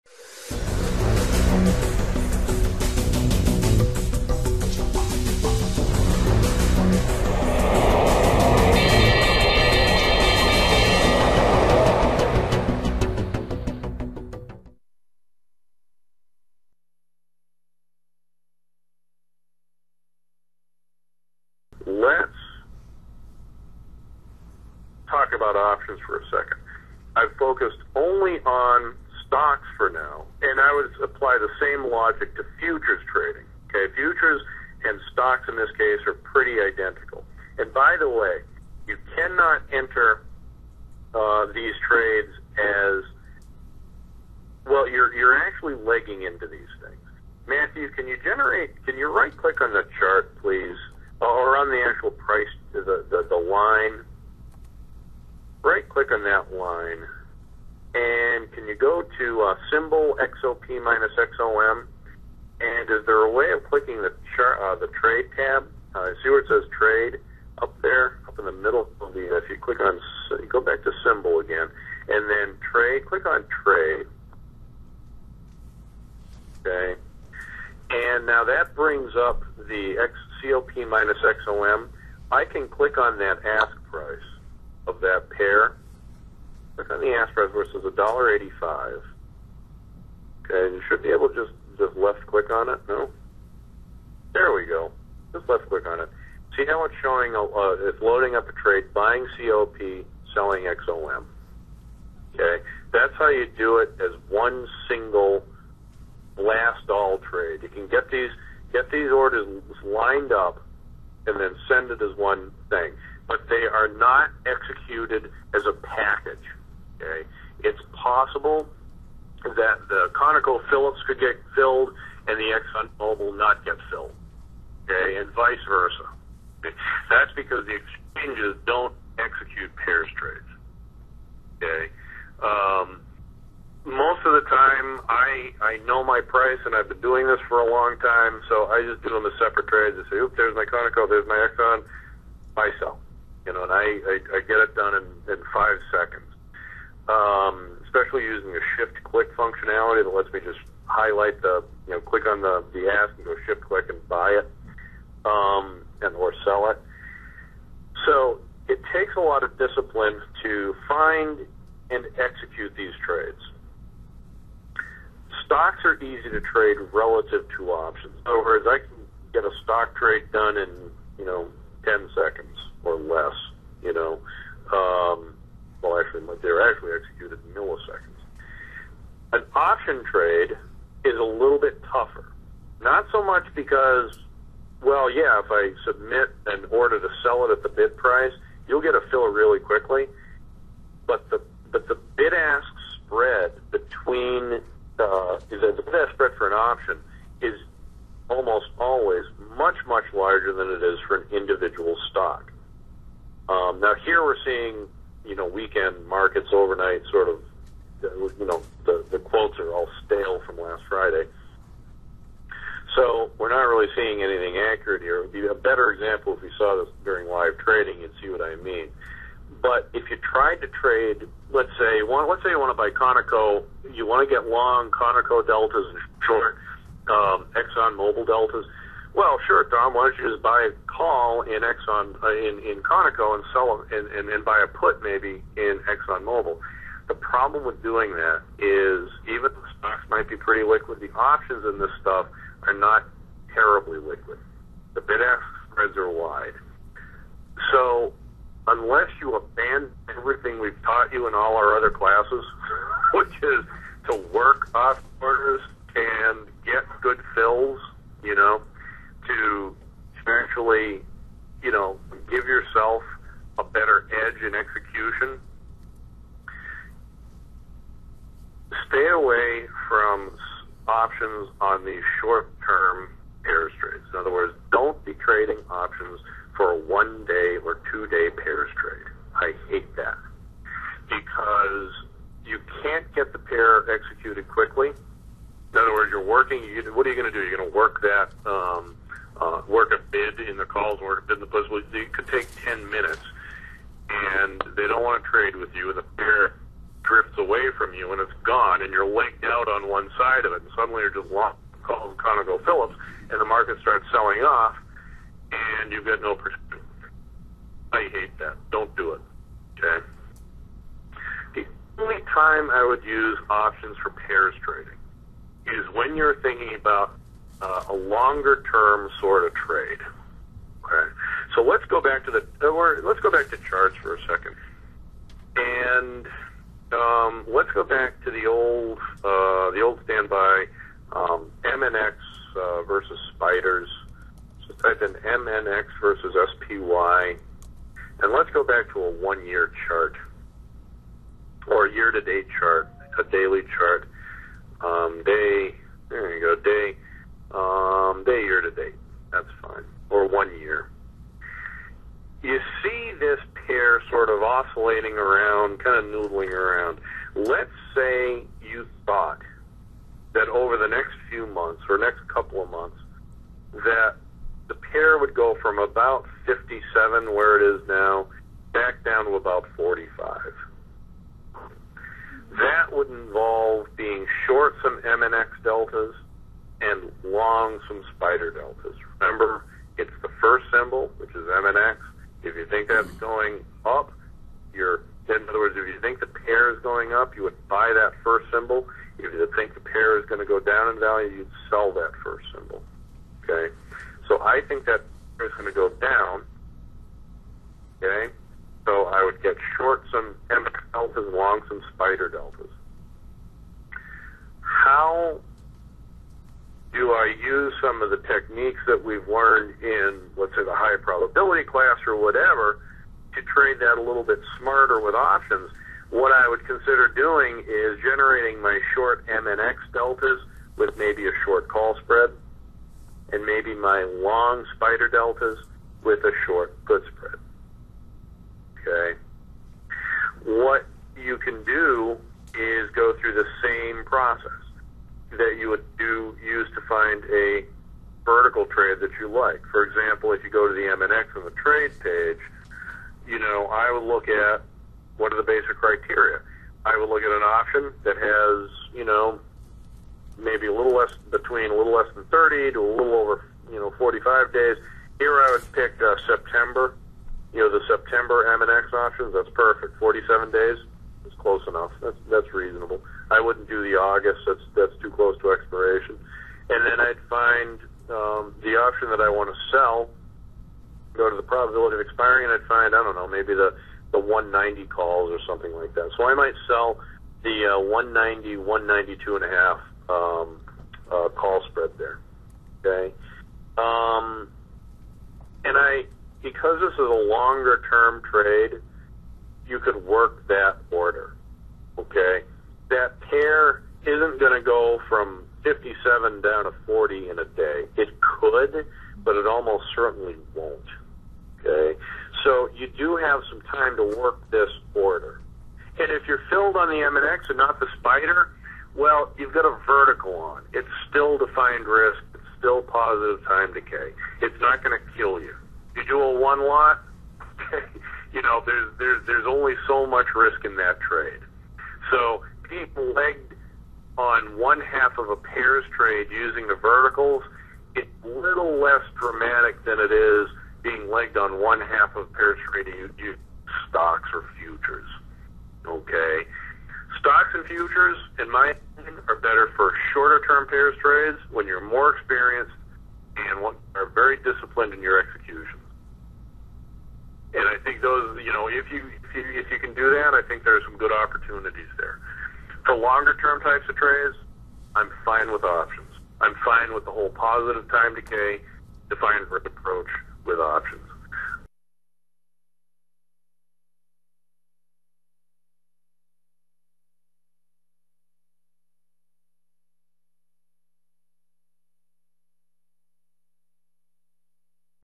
Pairs Trading: 6/9: Tom Preston: 08/02/08
Tom Preston from Thinkorswim Lecture on Pairs Trading to an audience of Probability Of Success Trading members on August 2nd, 2008